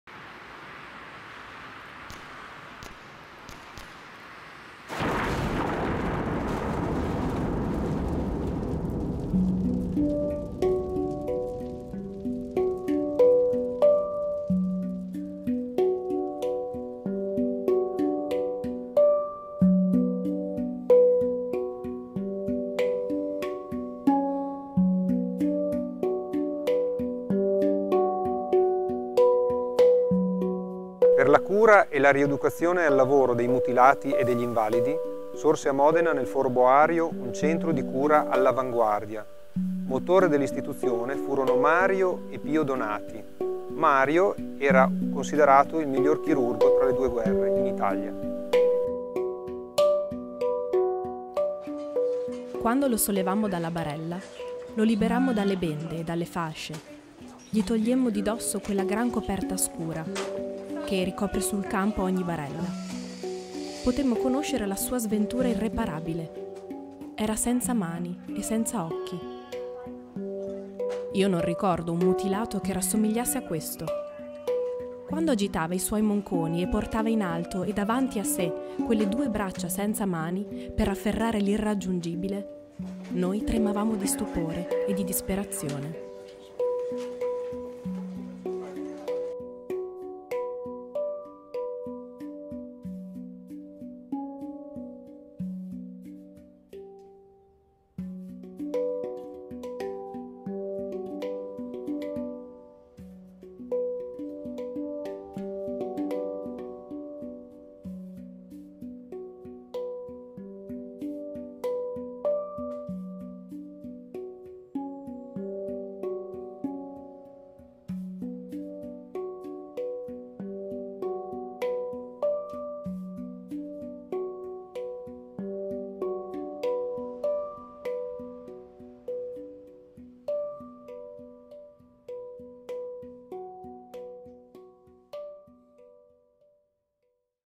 Modena sui passi della Grande Guerra - San Paolo
Un video per rievocare la Grande Guerra a Modena, dai segni della sua memoria come monumenti e lapidi, a luoghi di cui nel tempo si è perso il ricordo. Due voci, una del passato e una del presente, per rivivere quegli anni, in cui la città assieme al resto d’Europa entrava tragicamente nella modernità.